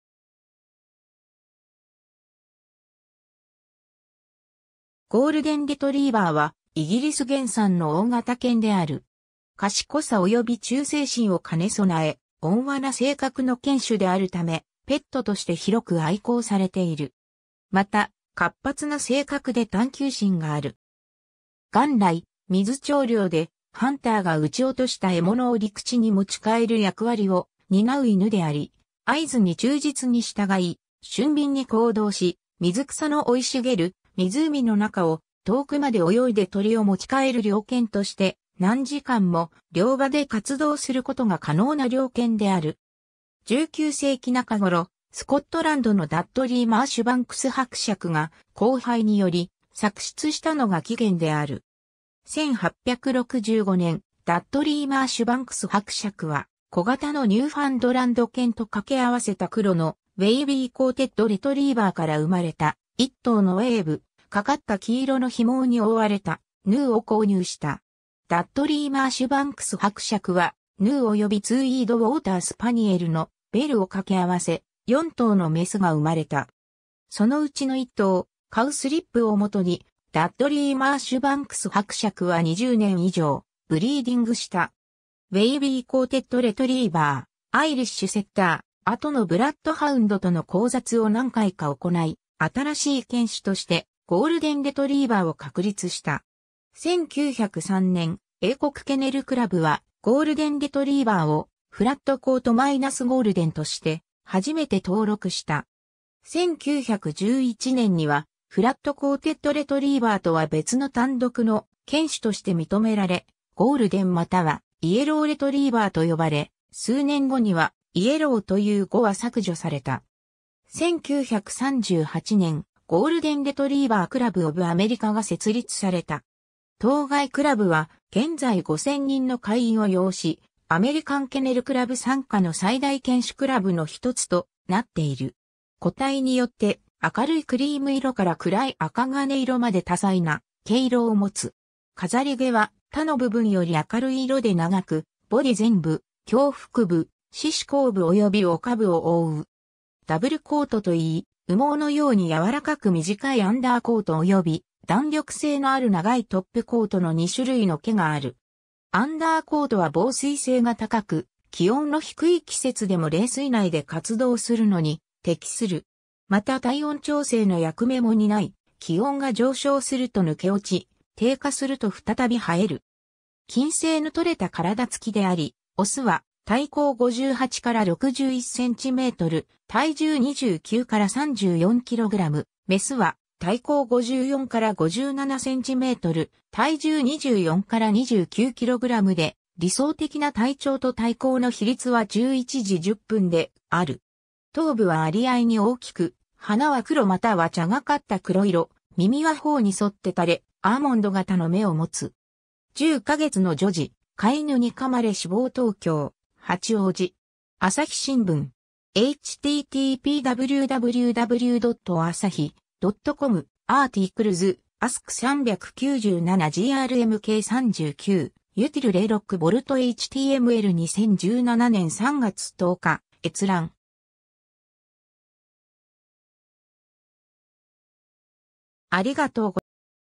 ゴールデン・レトリバー
ゴールデン・レトリーバー（ゴールデン・レトリバーとも、Golden Retriever）は、イギリス原産の大型犬である。
賢さ及び忠誠心を兼ね備え、穏和な性格の犬種であるためペットとして広く愛好されている。
また、活発な性格で探究心がある。
元来、水鳥猟でハンターが撃ち落とした獲物を陸地に持ち返る（＝retrieve）役割を担う犬であり、合図に忠実に従い、俊敏に行動し、水草の生い茂る湖の中を遠くまで泳いで鳥を持ち返る猟犬として、何時間も猟場で活動することが可能な猟犬である。
 19世紀中頃、スコットランドのダッドリー・マーシュバンクス伯爵（後のツウィードマウス卿）が交配により作出したのが起源である。
1865年、ダッドリー・マーシュバンクス伯爵は小型のニューファンドランド犬と掛け合わせた黒のウェイビーコーテッド・レトリーバーから生まれた、1頭のウェーブ掛かった黄色の被毛に覆われた「ヌー」を購入した。
ダッドリー・マーシュバンクス伯爵は「ヌー」及びツウィード・ウォーター・スパニエルの「ベル」を掛け合わせ、4頭の牝が生まれた。
その内の1頭「カウスリップ」を基に、ダッドリー・マーシュバンクス伯爵は20年以上ブリーディングした。
ウェイビーコーテッド・レトリーバー、アイリッシュ・セッター、後のブラッドハウンドとの交雑を何回か行い、新しい犬種としてゴールデン・レトリーバーを確立した。
 1903年、英国ケネルクラブはゴールデンレトリーバーを「フラットコート－ゴールデン」として初めて登録した。
1911年にはフラットコーテッド・レトリーバーとは別の単独の犬種として認められ、「ゴールデン又はイエロー・レトリーバー」と呼ばれ、数年後には「イエロー」という語は削除された。
 1938年、ゴールデン・レトリーバー・クラブ・オブ・アメリカ（GRCA）が設立された。
当該クラブは現在5000人の会員を擁し、アメリカンケネルクラブ（AKC）傘下の最大犬種クラブの1つとなっている。
 個体によって、明るいクリーム色から暗い赤金色まで多彩な毛色を持つ。
飾り毛は他の部分より明るい色で長く、ボディ前部、胸腹部、四肢後部及び尾下部を覆う。
 ダブルコートといい、羽毛のように柔らかく短いアンダーコート（下毛）及び弾力性のある長いトップコート（上毛）の二種類の毛がある。
アンダーコートは防水性が高く、気温の低い季節でも冷水内で活動するのに適する。
また体温調整の役目も担い、気温が上昇すると抜け落ち、低下すると再び生える。
 均整の取れた体付きであり、オスは体高58〜61cm、体重29〜34kg、メスは体高54〜57cm、体重24〜29kgで、理想的な体長と体高の比率は11：10である。
 頭部は割合に大きく、鼻は黒又は茶がかった黒色、耳は頬に沿って垂れ、アーモンド形の目を持つ。
^ “10カ月の女児、飼い犬にかまれ死亡　東京・八王子”. 朝日新聞. (2017年3月10日). 2017年3月10日閲覧。